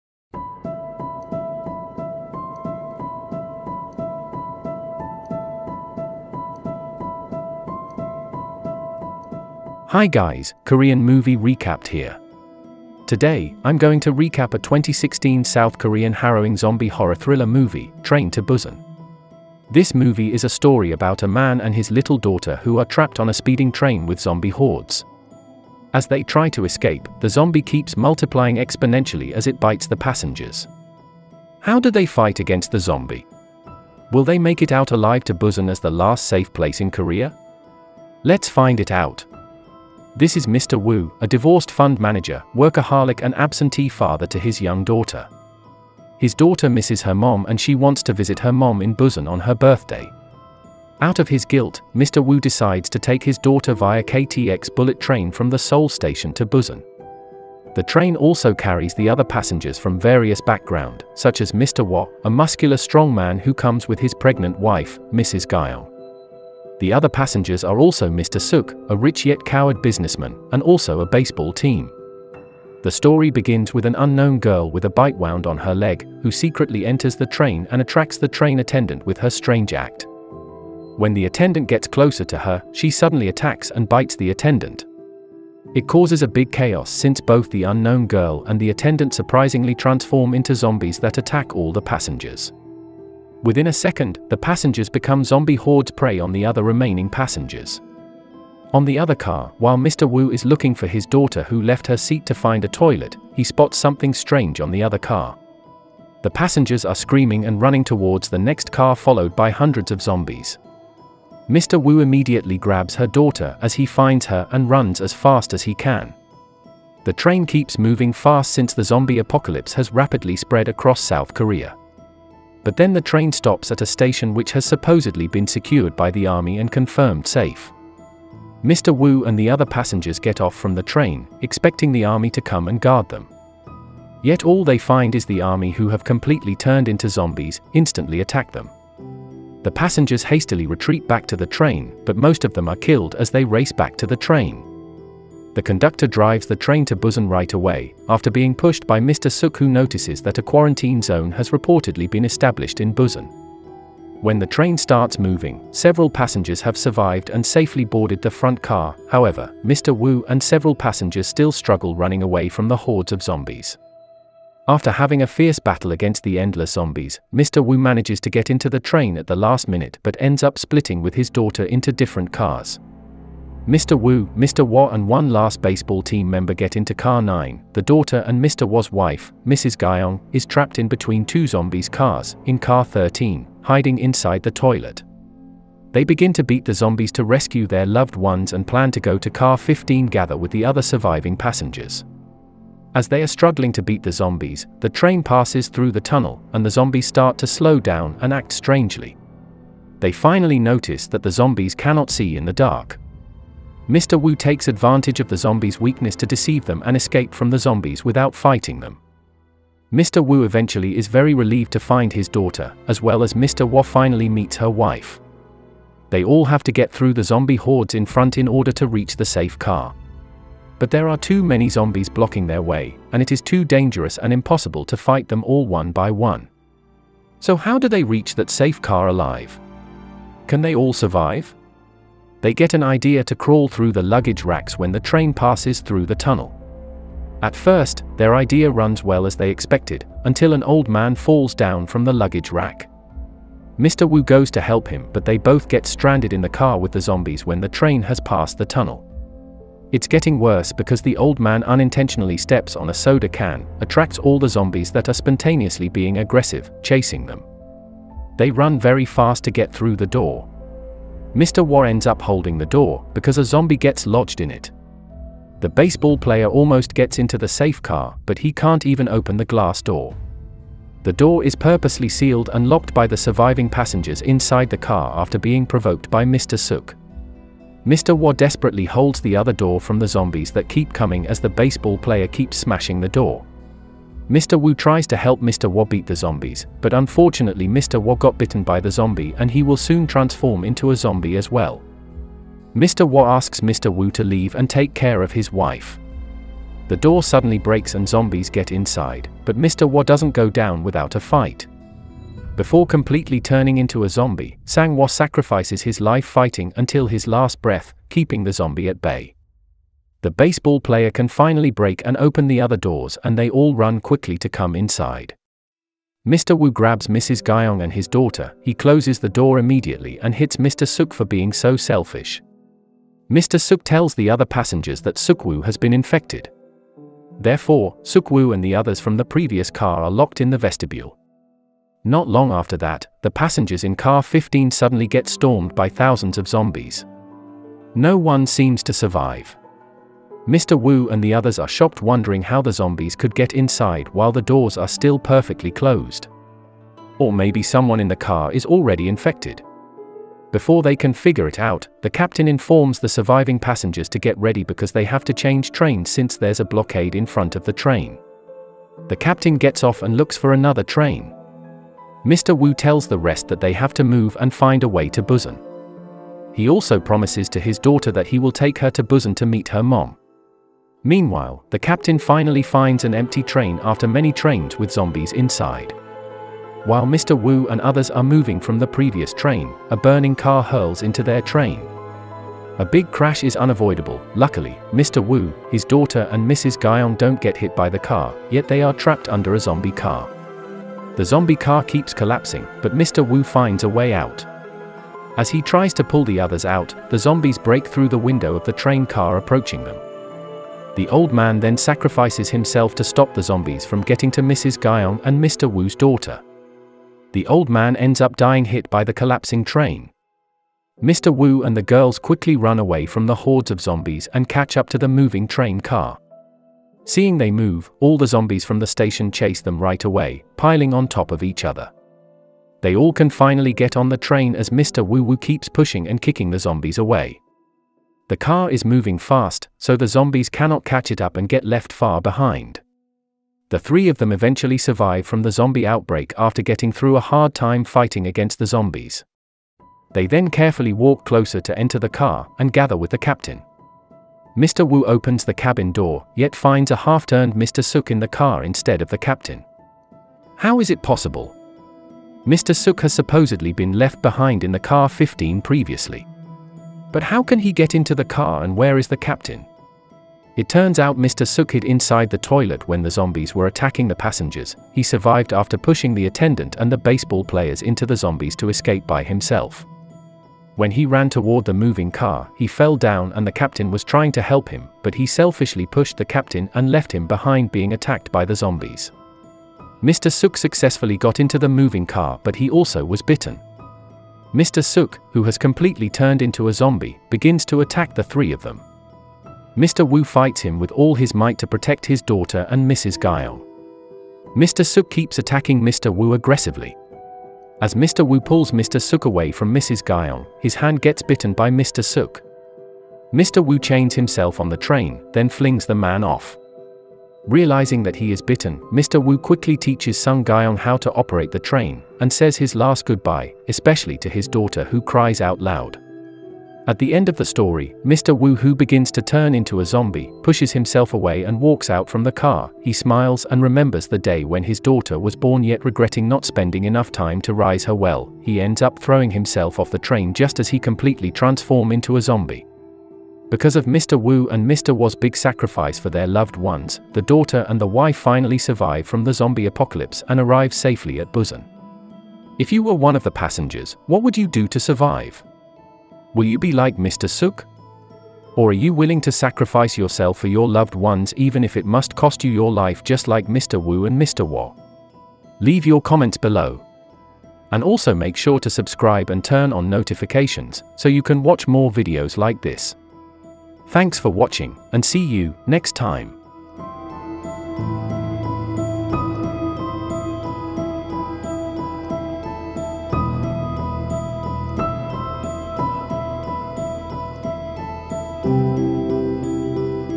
When Squid Game And Gangster Actors In A Movie. This Is What They Do! | Movie Story Recapped
A man and his daughter are enjoying their trip to Busan by KTX Bullet Train. Everything is going well until the passengers start to act strangely and attacking each other. The man and his daughter’s lives are now in danger. Will they survive from the chaos and make it to their last destination?

0:00 Opening
0:06 Movie Introduction
0:29 Movie Recap
8:20 Ending